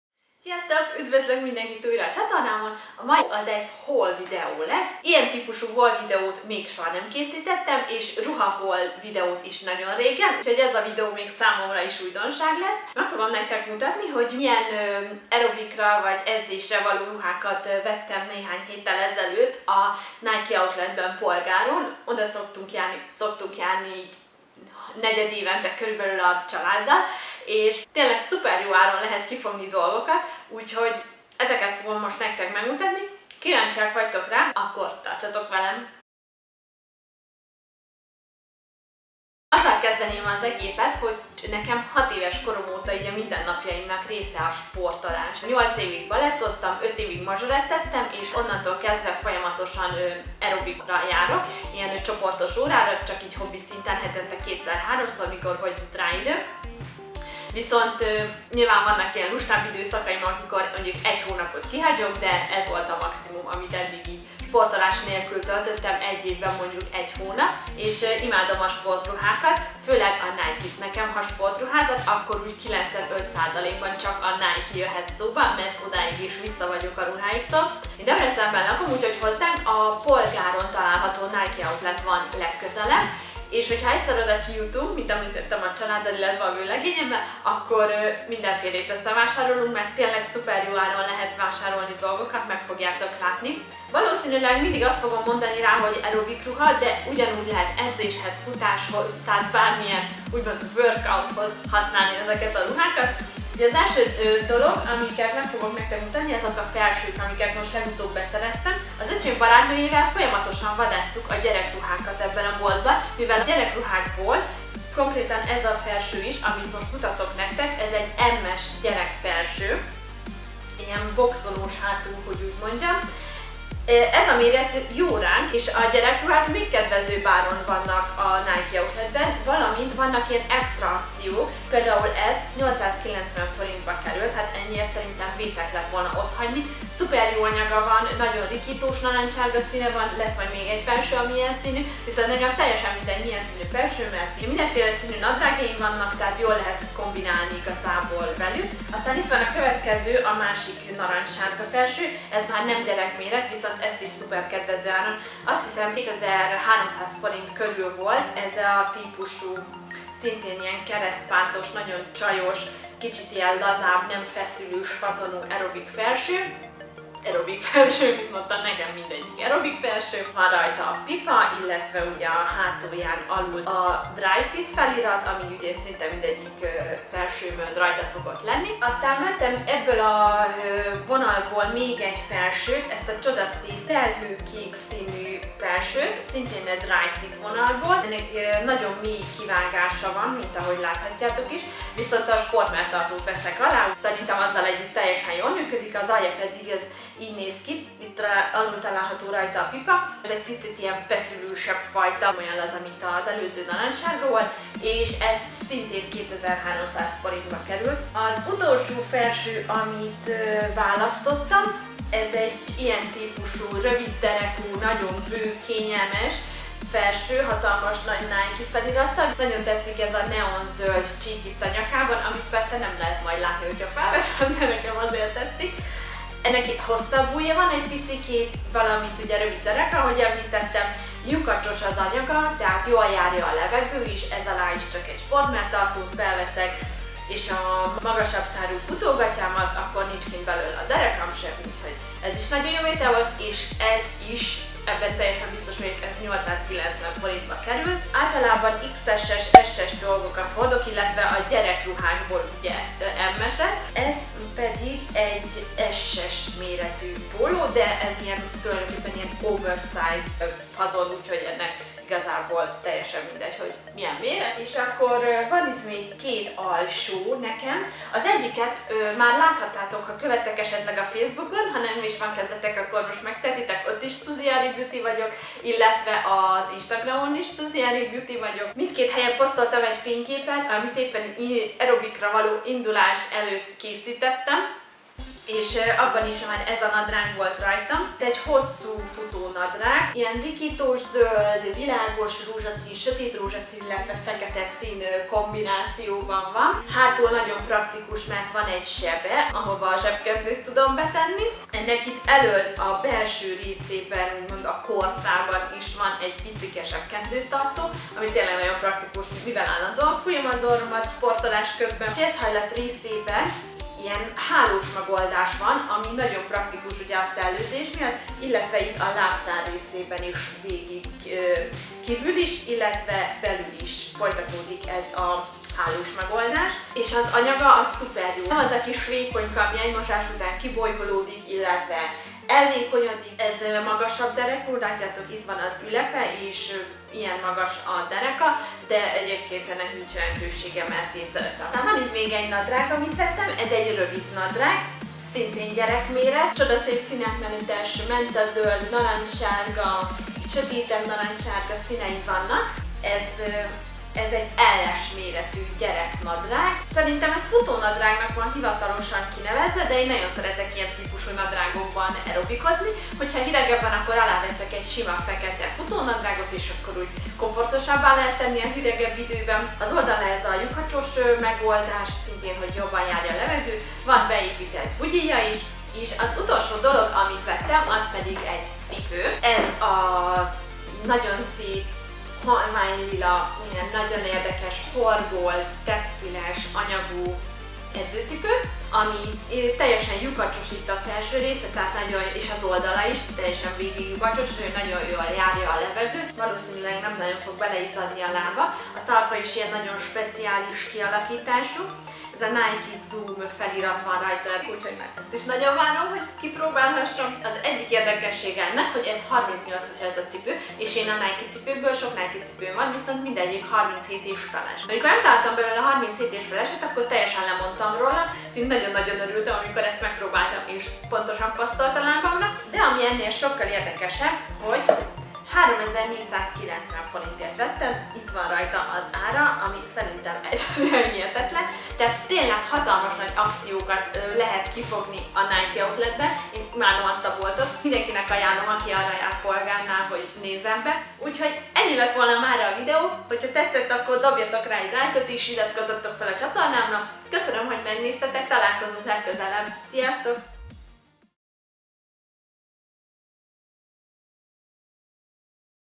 Aerobik/Workout Ruha Haul / NIKE
Sziasztok!
Itt egy olyan típúsú haul video amilyet még soha nem készítettem.
Erobik/fittness azaz workout ruhákat és cipőt zsákmányoltam az elmúlt hetekben A Polgáron található M3 Outletben található NIKE boltból :), és ezeket fogom nektek ma megmutatni! ;)
Ha esetleg érdekel titeket hogy merre van és milyen boltok vannak még az Outletben akkor lentebb megtaláljátok a linket! ;)

Mickey egeres póló: Berska
Rúzs: MeNow hosszantartó szájfény - 12
Óra: Michael Kors